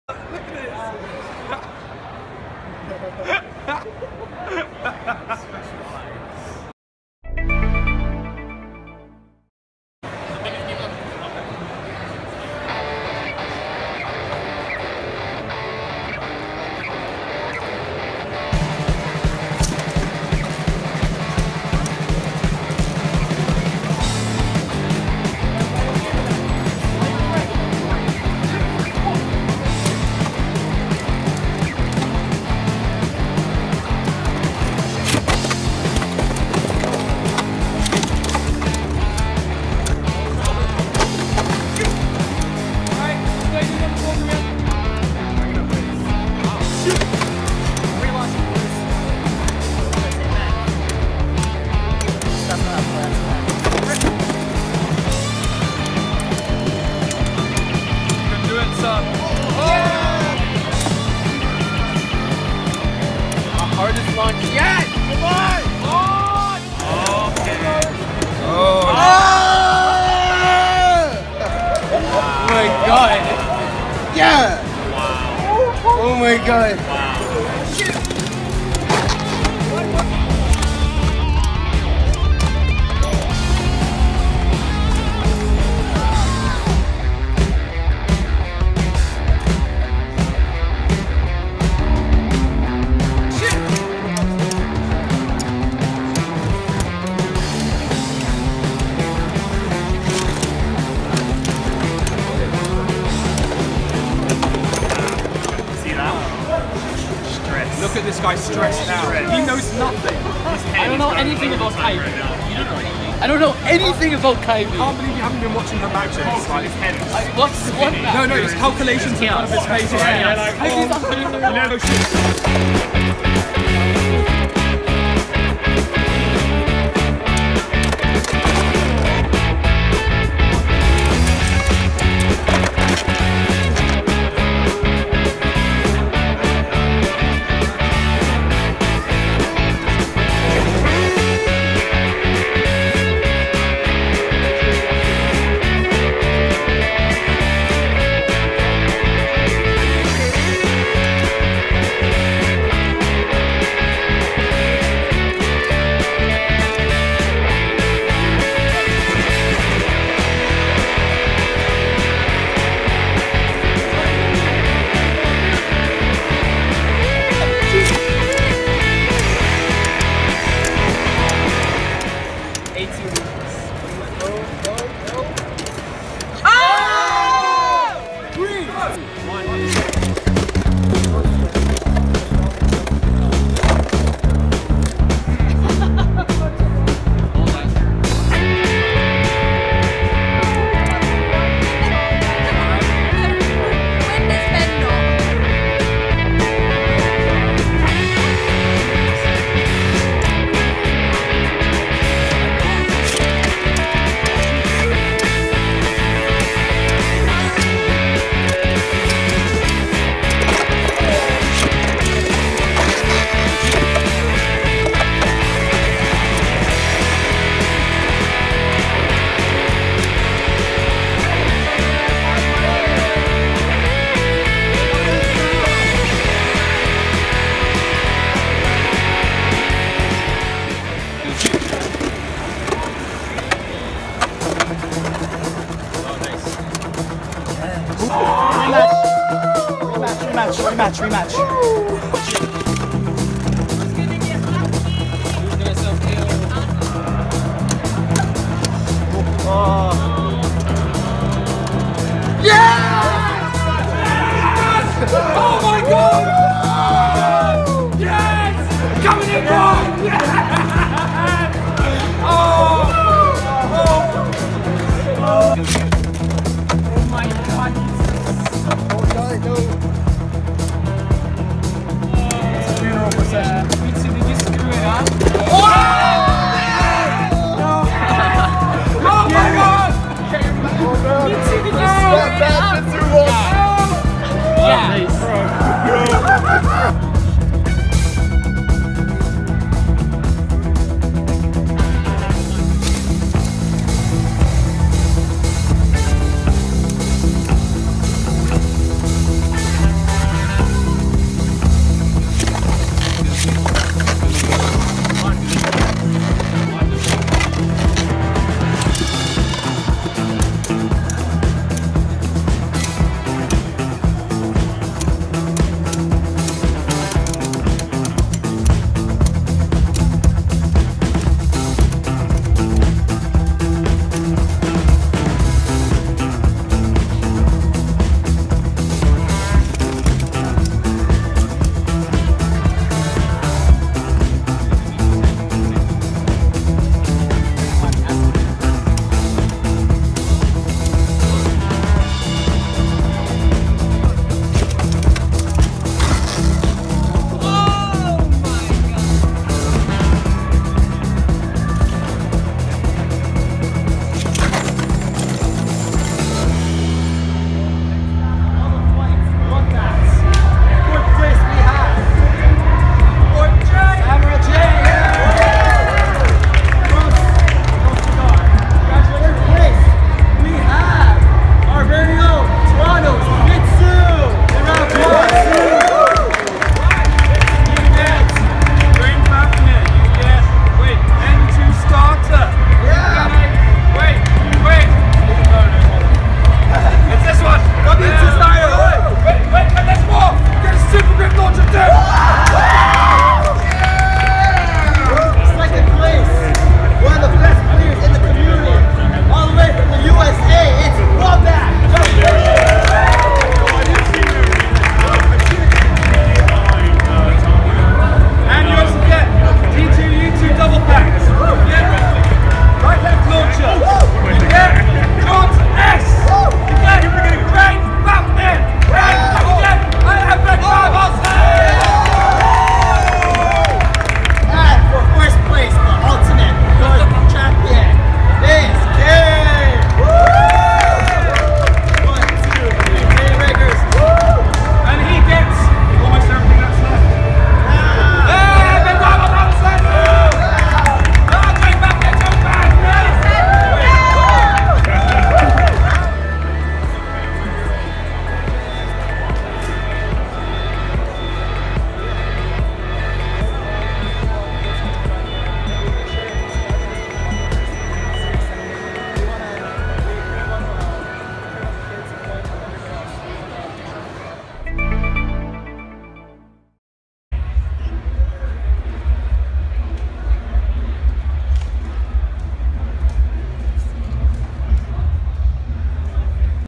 Beyblade Burst Tournament - BEYBLADE NORTH @ Anime North 2017, Toronto, Canada (ベイブレードバースト大会)
Beyblade Channel is presented to you all by the World Beyblade Organization.

On May 27th, 2017 in Toronto, Canada, the best Bladers from Canada, the US, and UK experienced the second of three days of high-speed battling action and Beyblade giveaways at BEYBLADE NORTH 2017! Using Beyblades from the explosive new series–Beyblade Burst–over 40 Bladers competed for a generous pool of prizes provided by the WBO, Anime North, and Hasbro, and for the title of BEYBLADE NORTH champion!

Beyblade tournaments are regularly hosted by the World Beyblade Organization around the globe, and you can even start up a tournament scene right where you live!